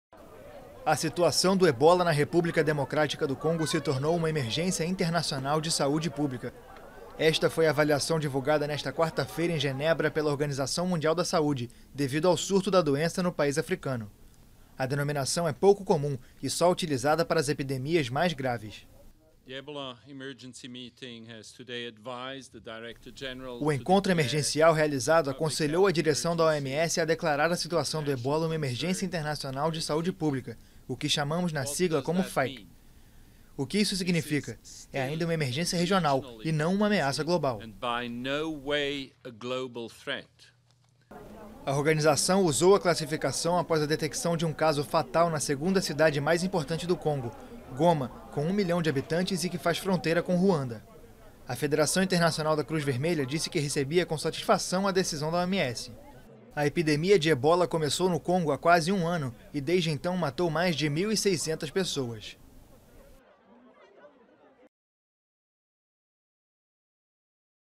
OMS adverte sobre Ebola no Congo | AFP
A Organização Mundial da Saúde (OMS) declarou nesta quarta-feira o surto de ebola na República Democrática do Congo uma ‘emergência de saúde pública de interesse internacional’. A denominação é pouco comum e só utilizada para as epidemias mais graves.